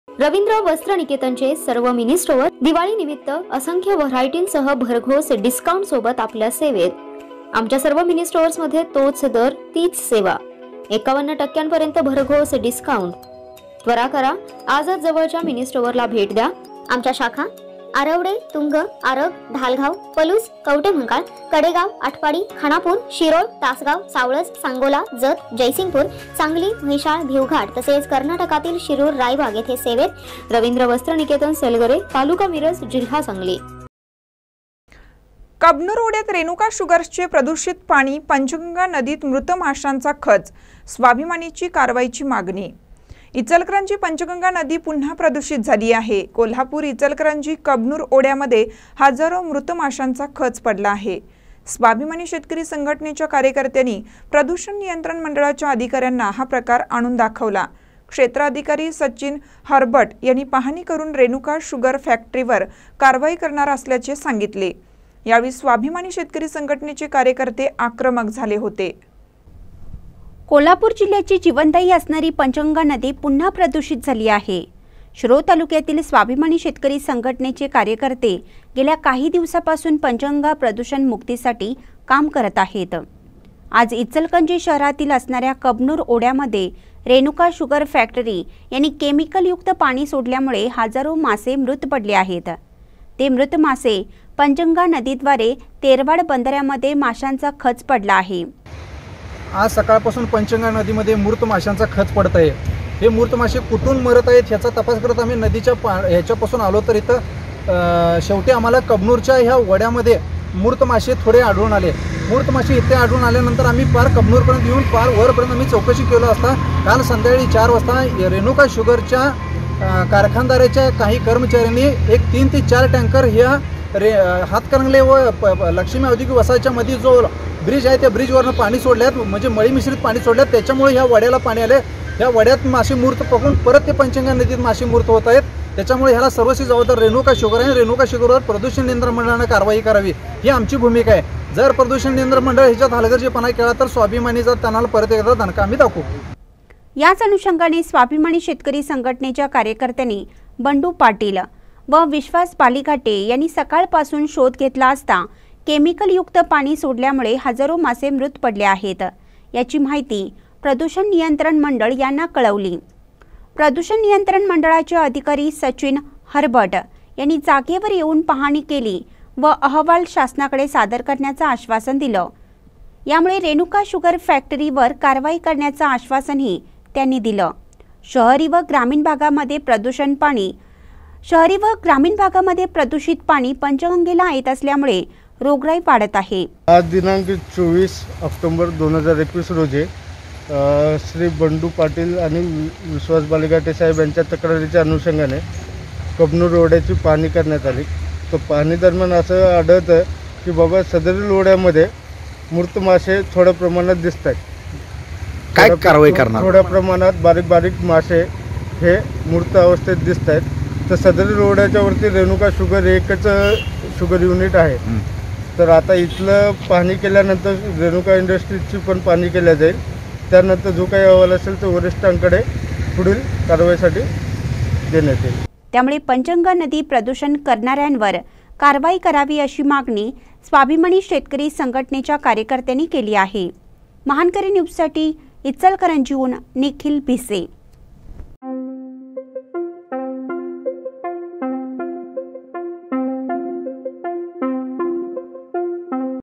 कबनूर ओढ्यात रेणुका शुगर्सचे प्रदुषित पाणी, पंचगंगा नदीत मृत माशांचा खच, स्वाभिमानीची कारवाईची
कबनूर ओढ्यात रेणुका शुगर्सचे प्रदुषित पाणी, पंचगंगा नदीत मृत माशांचा खच, स्वाभिमानीची कारवाईची मागणी